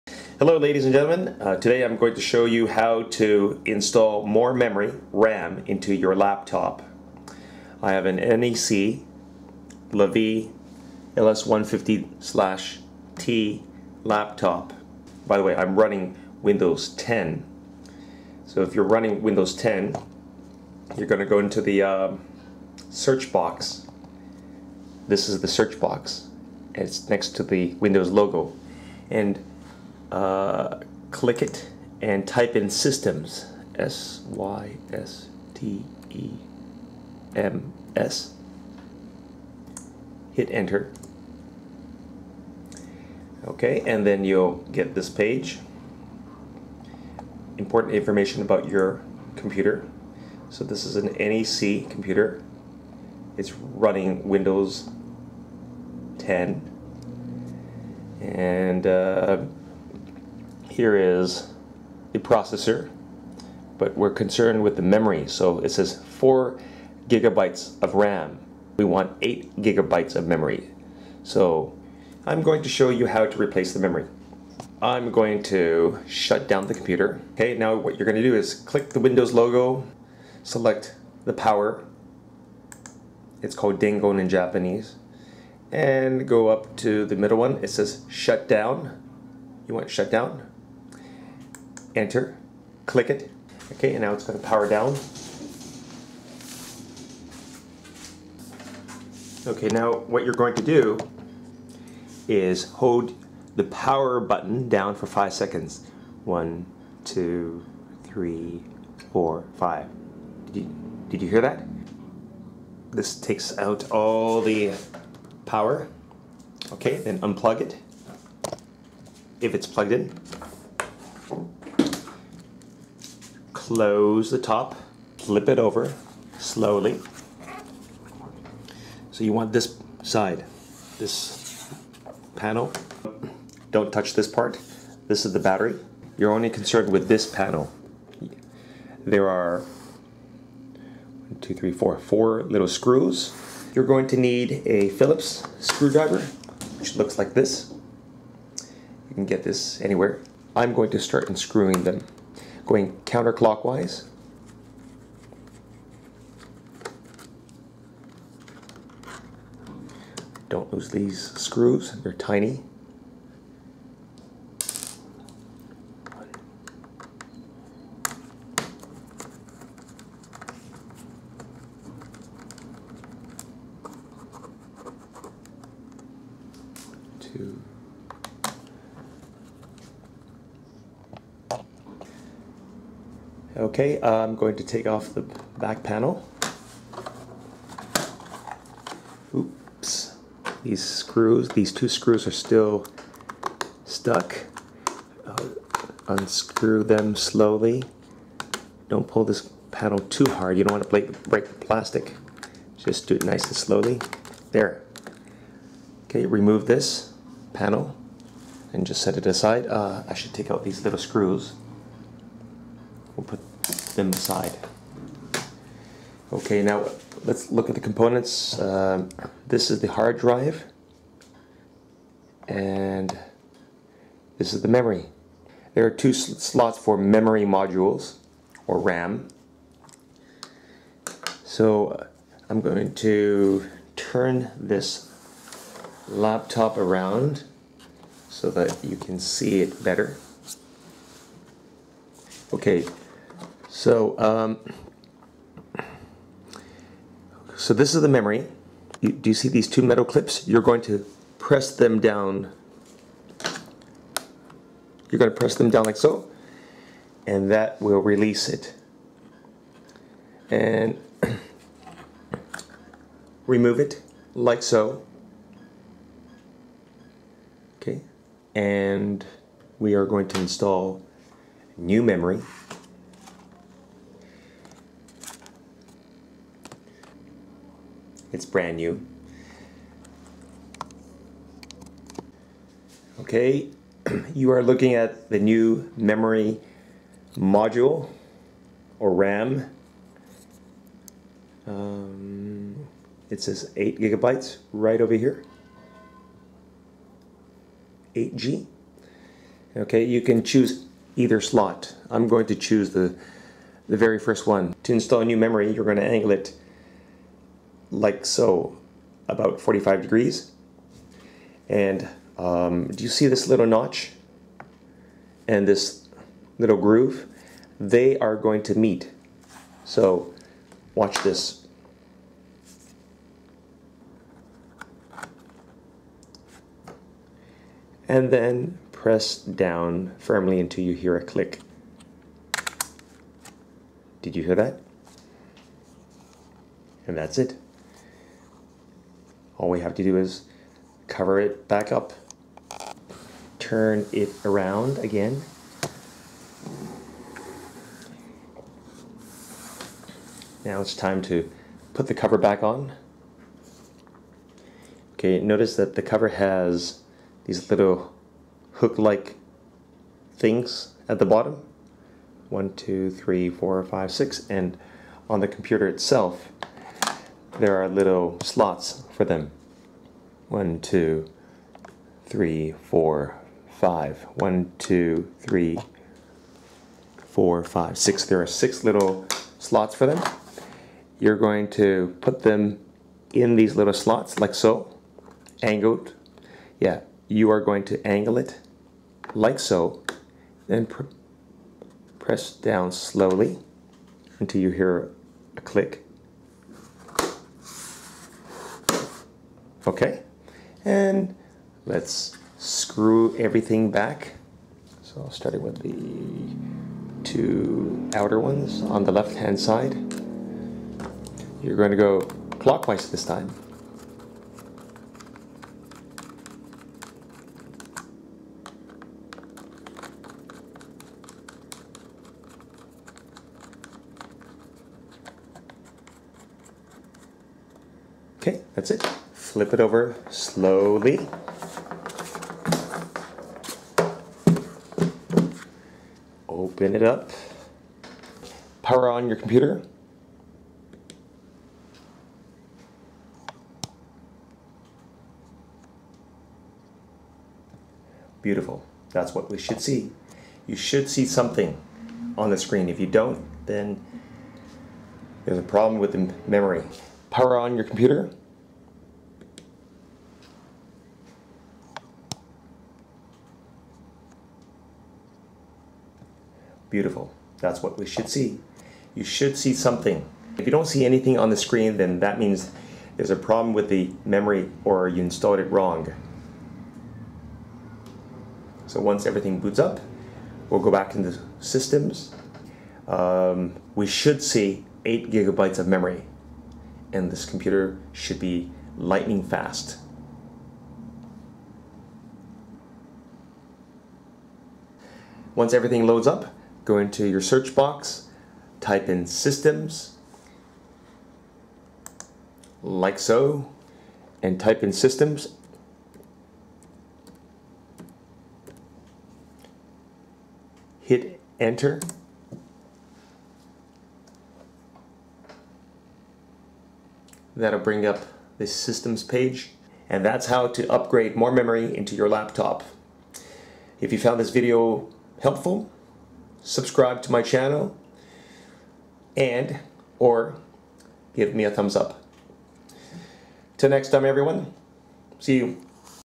How to Install Memory sticks (RAM) to your computer
I will show you how to install computer memory sticks RAM to your pc.